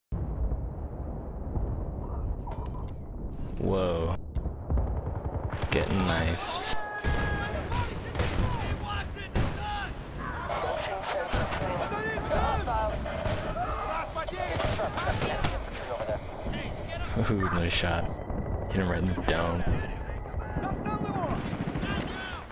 Rekkit "Wreck It" Smashenheimer Calls It Like He Sees It
I'm still amazed that I can record this game while playing it on my computer. Featuring unwitting commentary from one of the buddies.